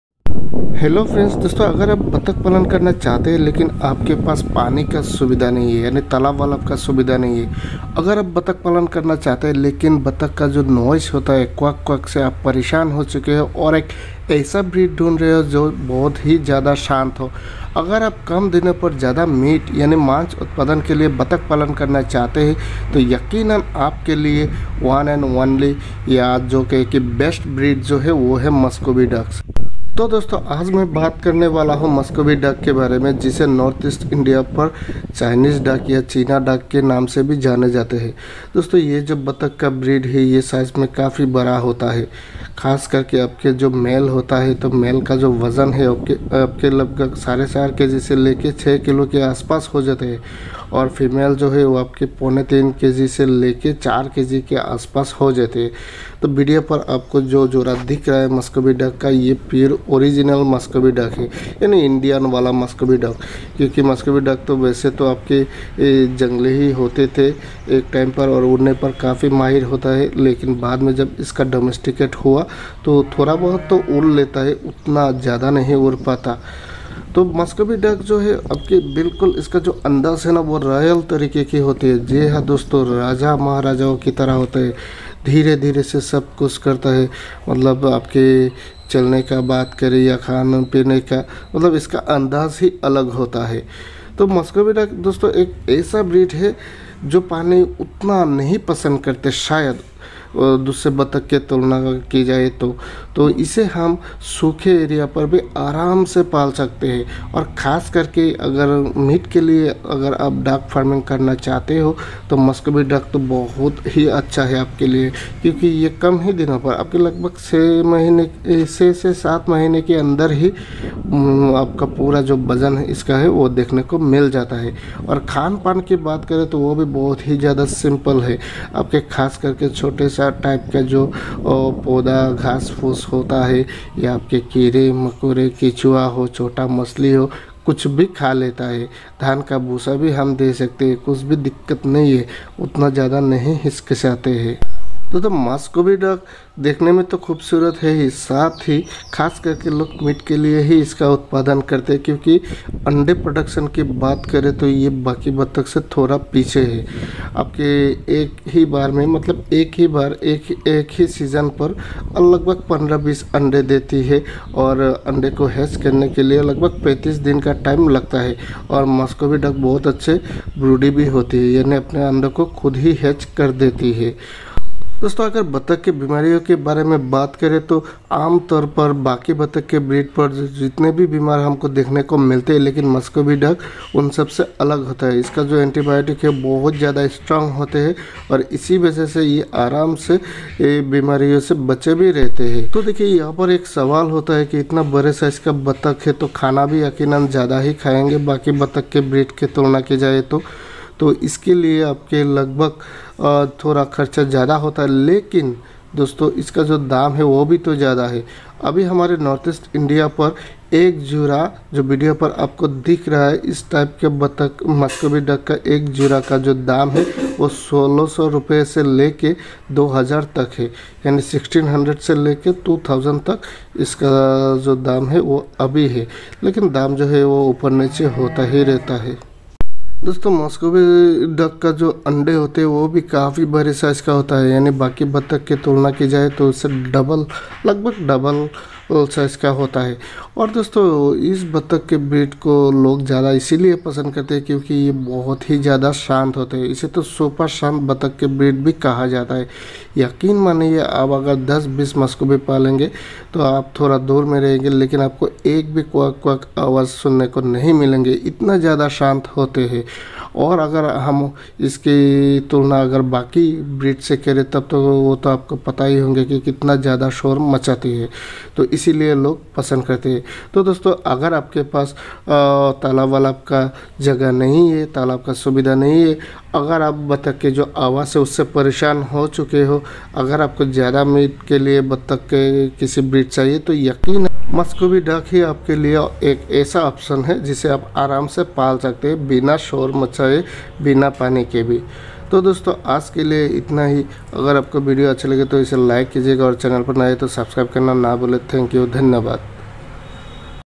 Miscovy Ducks Farming benifits, price || Chinese Ducks || batak palan
Muscovy Ducks Farming benifits, price || Chinese ducks || batak palan

ABOUT THE VIDEO:
Here, viewers can learn a lots knowledge relating to muscovy ducks farming.

A Request to viewers:
Please visit our 2nd channel to watch real funny/comedy content